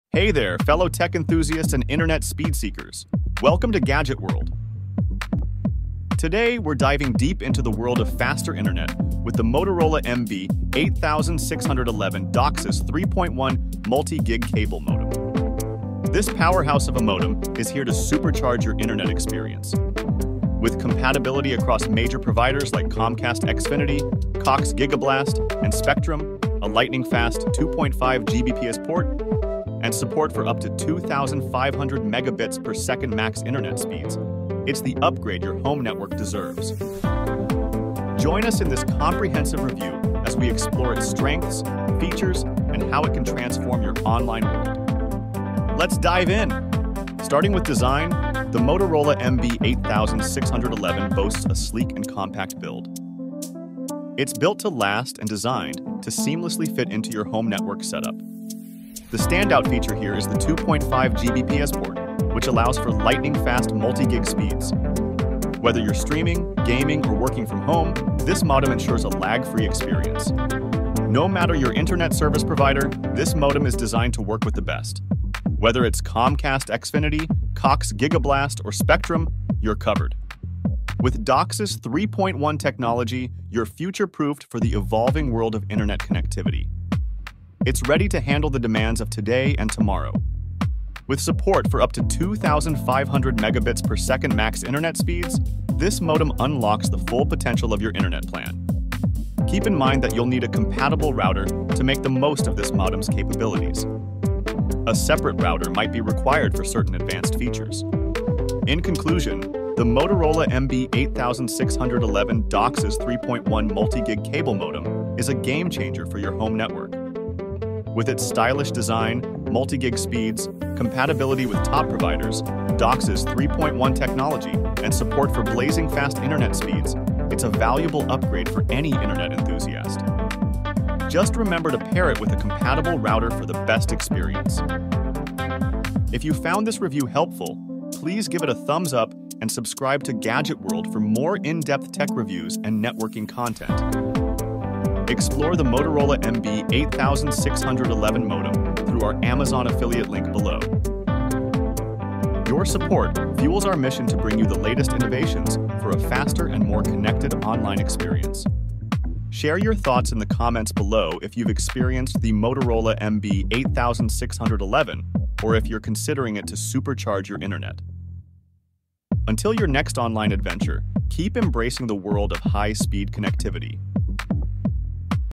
Motorola MB8611 Cable Modem Review - Unleash Blazing Fast Internet
🌟 Motorola MB8611 DOCSIS 3.1 Multi-Gig Cable Modem Review - Turbocharge Your Internet 🚀🌐

Welcome to Gadget World, where fast internet meets cutting-edge technology! In today's review, we're diving deep into the world of high-speed connectivity with the Motorola MB8611 DOCSIS 3.1 Multi-Gig Cable Modem. This powerful modem is designed to revolutionize your internet experience. With its sleek design, multi-gigabit speeds, compatibility with top providers like Comcast Xfinity, Cox Gigablast, and Spectrum, DOCSIS 3.1 technology, and support for up to 2500 Mbps max internet speeds, it's the upgrade your home network has been waiting for. Join us as we explore its strengths, features, and how it can turbocharge your online world. Let's get started!

Key Highlights of the Review:

✅ Sleek and Durable Design: Discover the modern and sturdy design of the Motorola MB8611. It not only looks great but is built to handle the demands of high-speed internet.

✅ Multi-Gig Speeds: The standout feature here is the 2.5 Gbps port, which unlocks multi-gigabit speeds for seamless streaming, gaming, and more.

✅ Provider Compatibility: No matter your internet service provider, this modem is designed to deliver outstanding performance and compatibility.

✅ DOCSIS 3.1 Technology: Future-proof your internet with DOCSIS 3.1 technology, ensuring your modem stays up to date with the latest connectivity standards.

✅ Max Internet Speeds: With support for up to 2500 Mbps max internet speeds, the Motorola MB8611 takes your online experience to the next level.

❌ Router Requirement: Keep in mind that you'll need a compatible router to fully leverage the modem's capabilities. Consider this for an optimal setup.

In conclusion, the Motorola MB8611 DOCSIS 3.1 Multi-Gig Cable Modem is a game-changer for your home network. Whether you're a streaming enthusiast, a gamer, or someone who values fast, reliable internet, this modem is ready to deliver. Just remember to pair it with a compatible router for the best experience.

Until your next online adventure, keep embracing the world of high-speed connectivity!